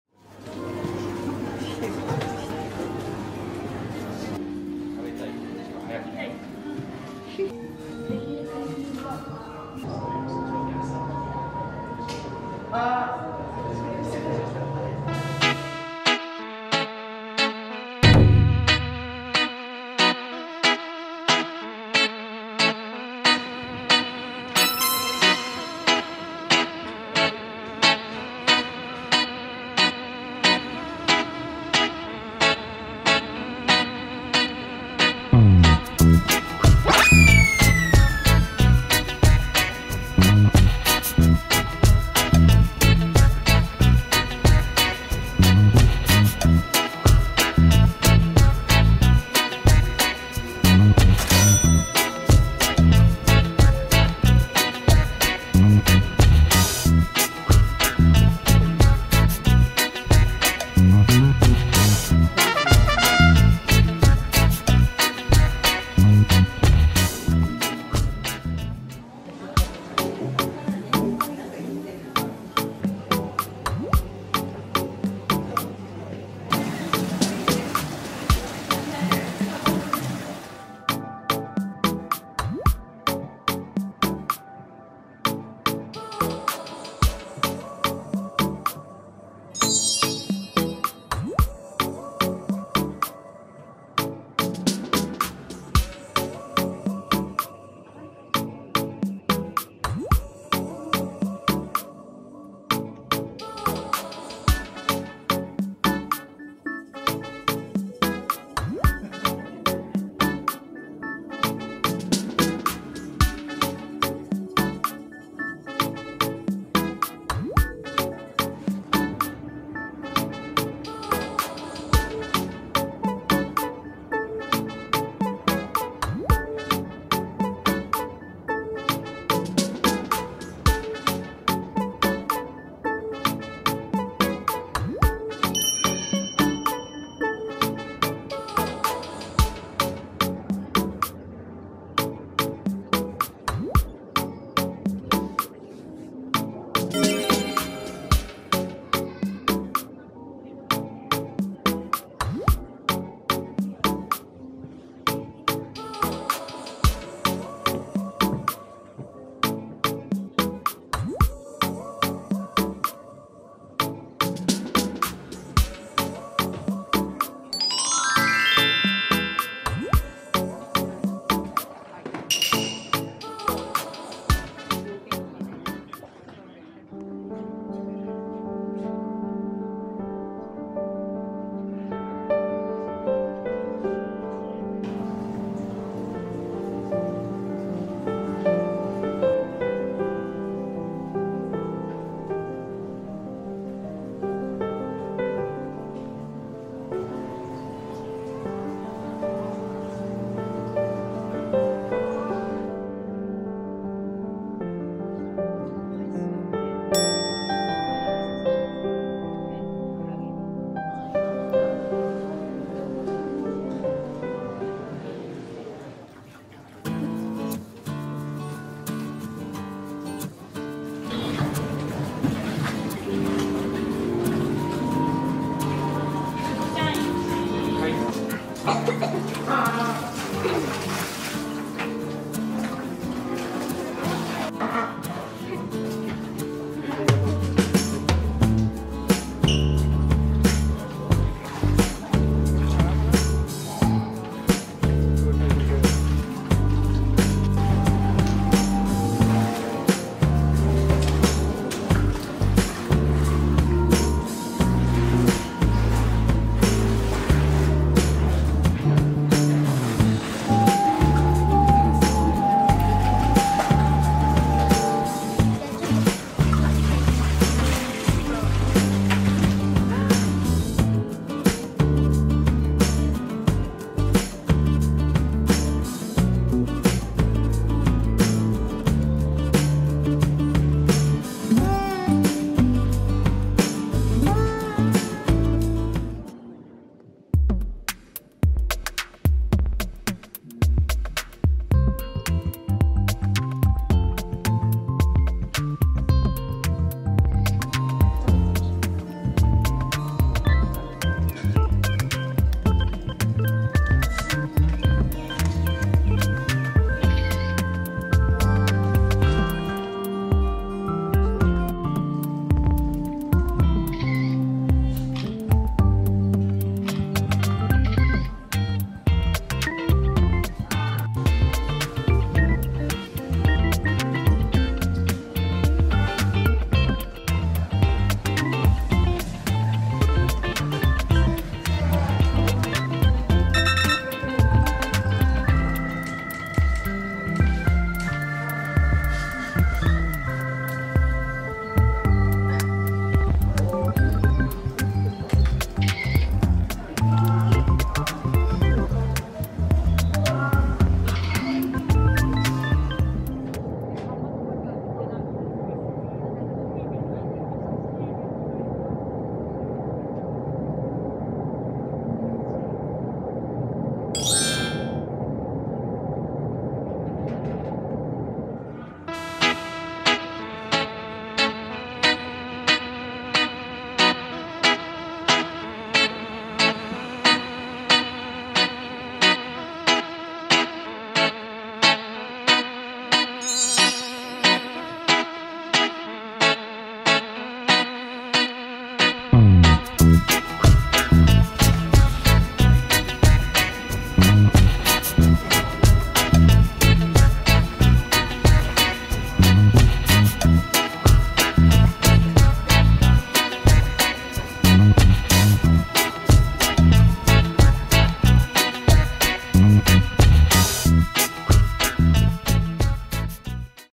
[安らぎの空間]AOAO SAPPORO 行ってきた
2023年7月20日　札幌の街のど真ん中、狸小路のお隣にオープンしたmoyukuSAPPORO。その4階〜6階に水族館が入りました(・∀・)
オープン直後の混雑は避けて、落ち着いたころに見学に行ってきましたよ。

今までの水族館とは違う、令和に生まれた落ち着きのある空間を演出する新しい水族館。学びはもちろん癒やしの空間としても遊びに行く価値は十分かと(^^)